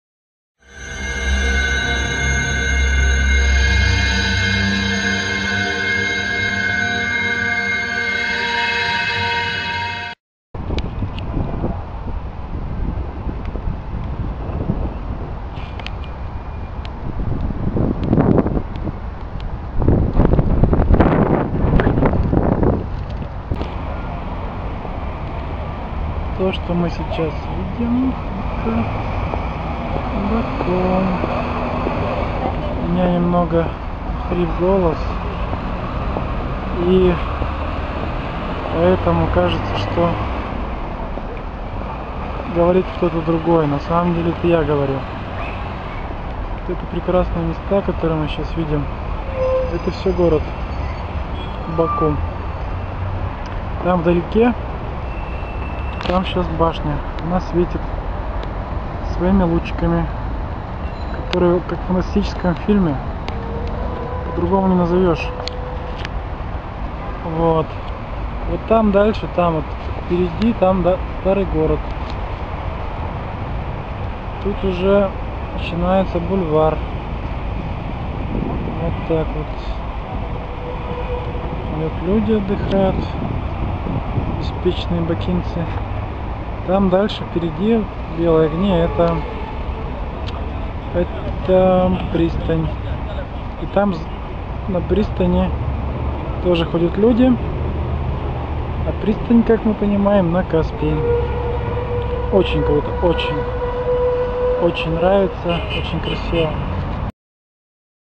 Прекрасный город Баку в декабре. Мое путешествие по Азербайджану.
Прекрасный город Баку, в котором прошло большая часть моего  путешествия по Азербайджану.